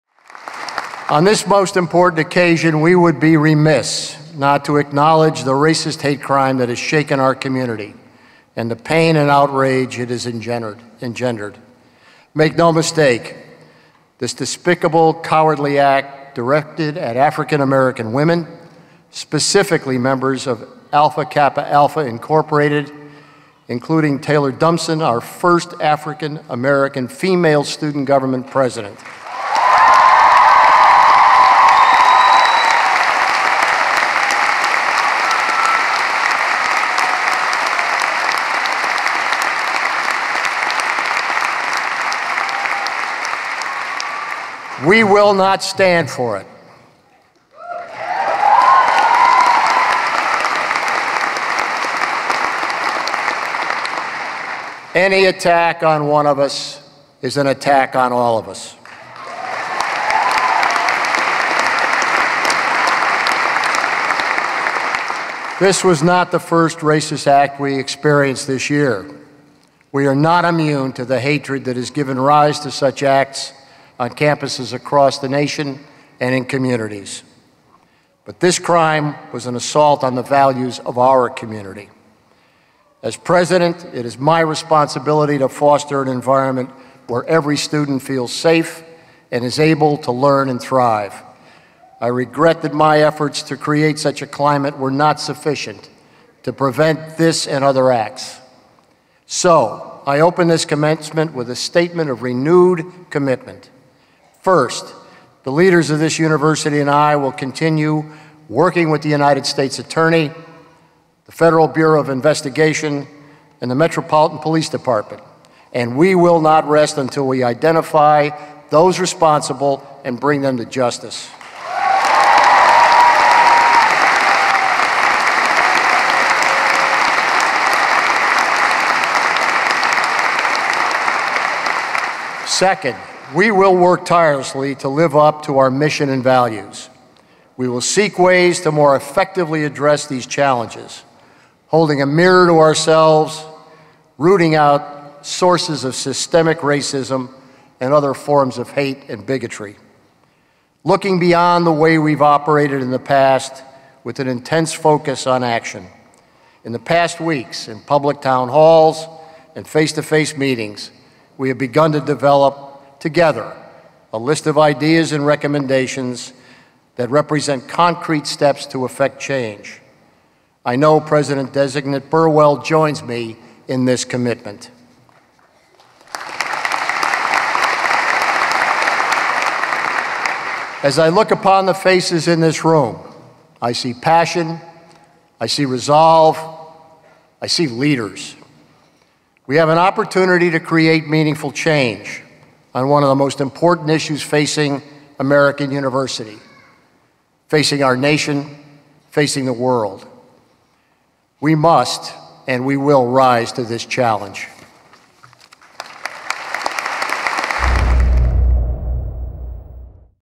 American University President Kerwin Commencement Address on Hate
For the latest updates and information on how American University is responding to the hate crime committed on its campus,

American University President Neil Kerwin addressed the recent racist hate crime on campus during his May 13 commencement speech. President Kerwin shared his outrage and regret. He also renewed the university’s commitments to bringing the perpetrators to justice, living up to its mission and values, and creating meaningful change on one of the most important issues facing American University, the nation, and the world.